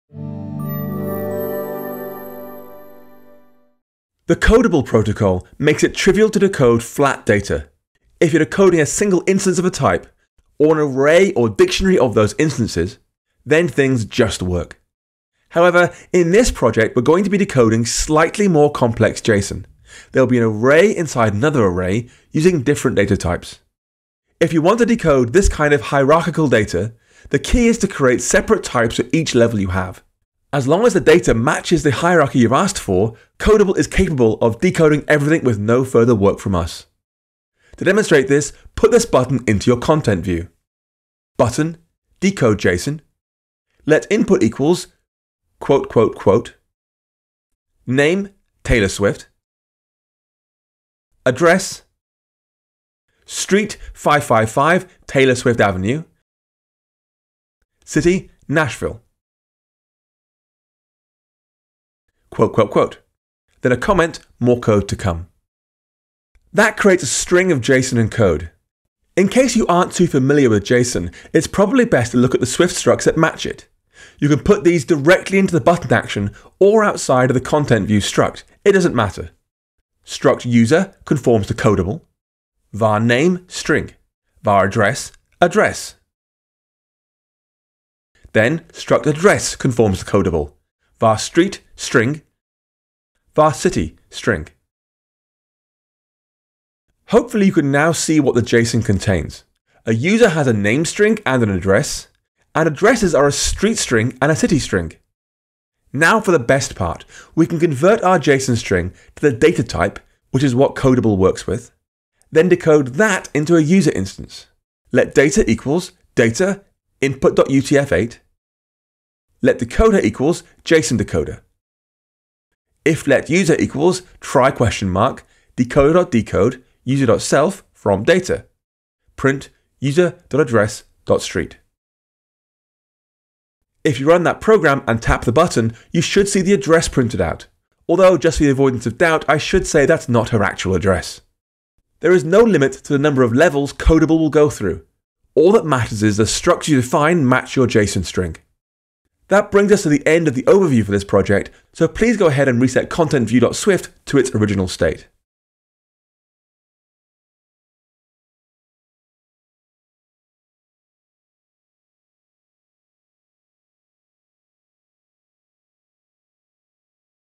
(OLD) Working with hierarchical Codable data – Moonshot SwiftUI Tutorial 4/10
In this project we build a multi-screen user interface for letting users explore the history of the Apollo space program.